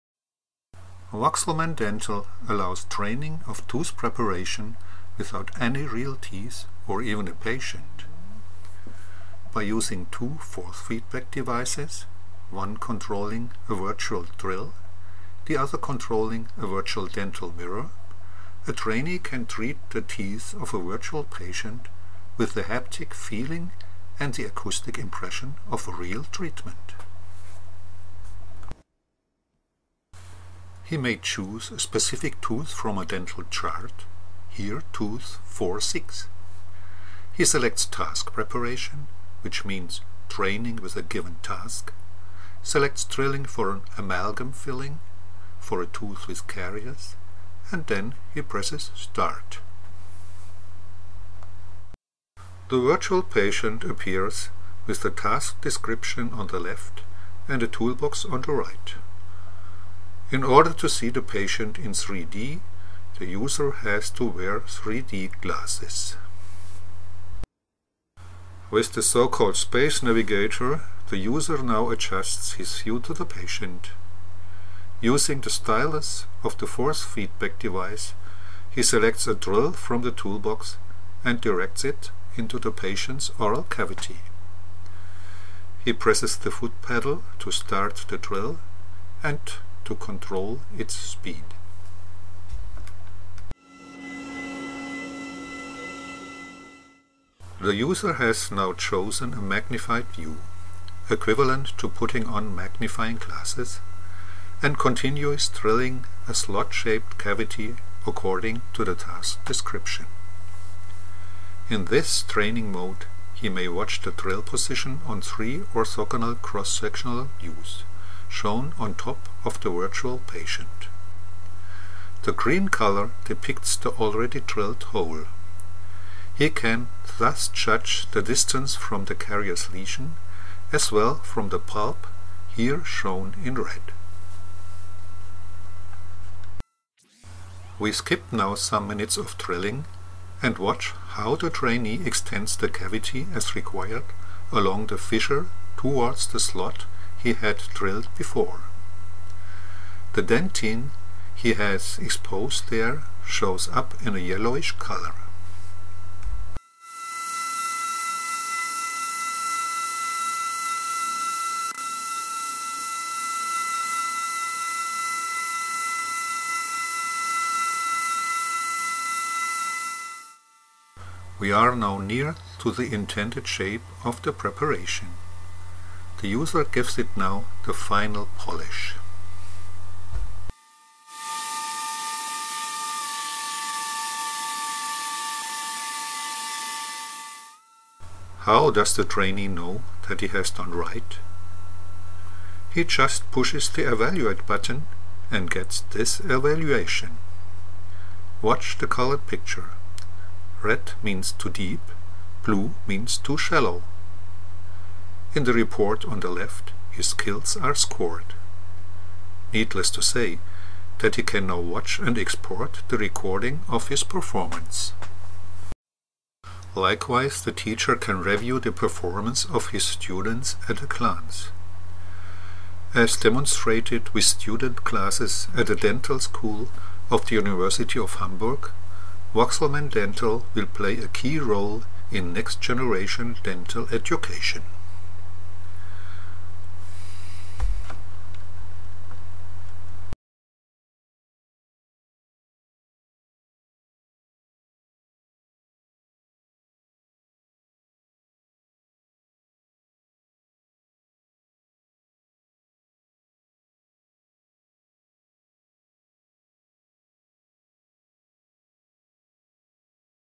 Cavity Preparation with Voxel-Man Dental
Demonstration VOXEL-MAN Dental, a novel tool for virtual reality training in dentistry, Besides providing a realistic visual, haptic and acoustic feeling it allows an automatic skills assessment.